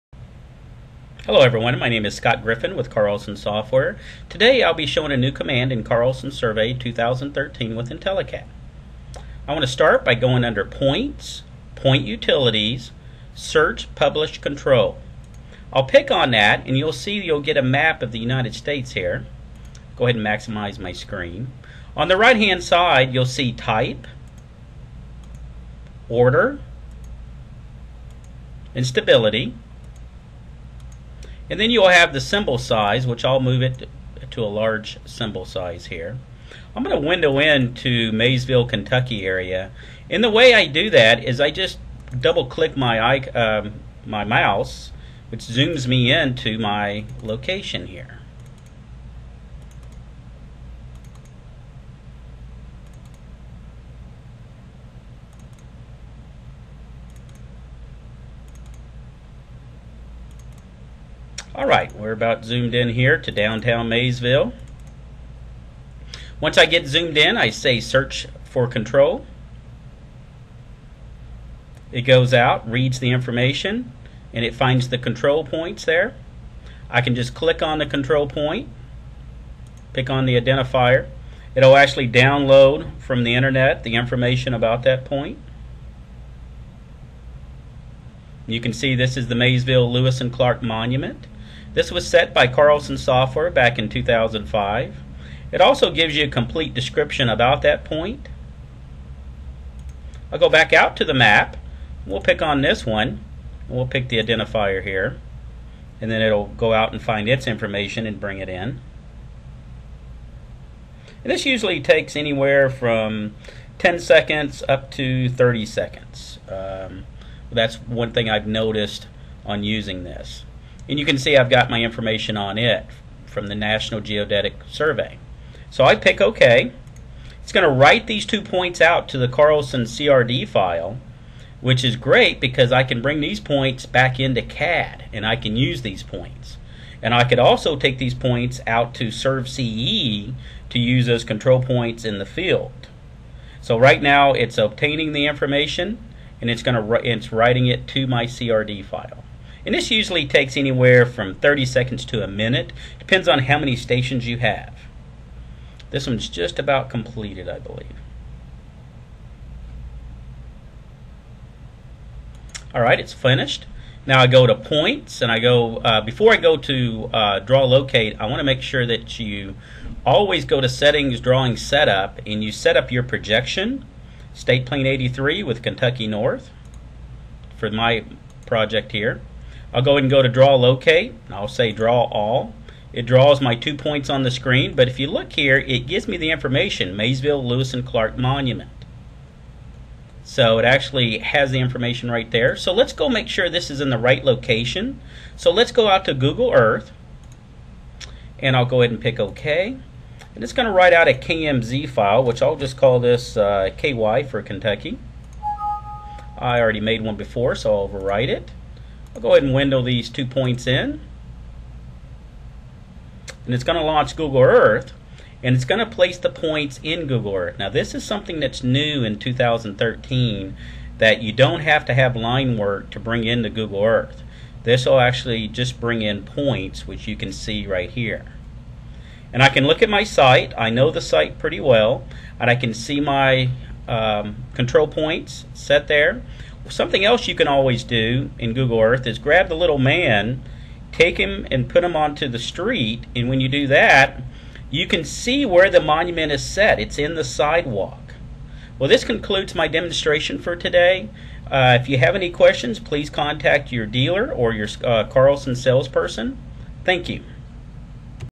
Search Published Control, Carlson Survey 2013
This tutorial shows how to search for published control points and view the National Geodetic Survey (NGS) data sheets associated with them in Carlson Survey 2013 with IntelliCAD.  Presented by Scott Griffin.